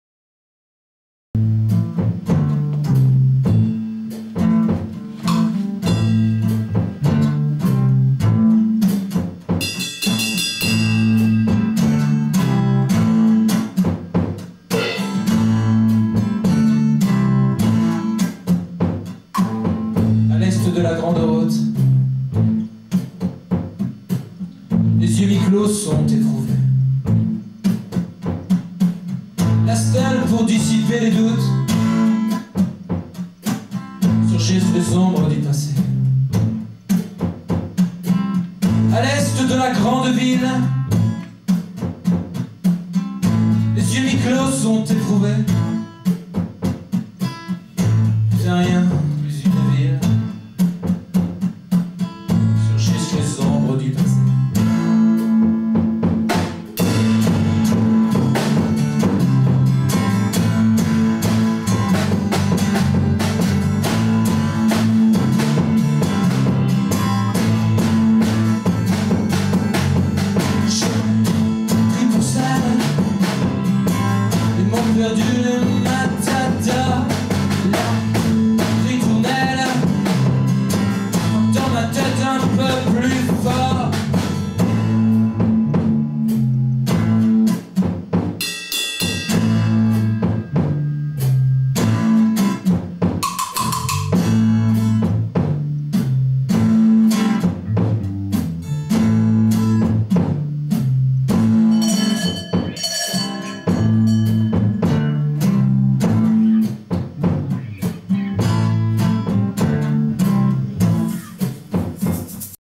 repette mamz'elle poulie
répetition du groupe Mam'Zelle Poulie à l'ancienne salle de musique de Golbey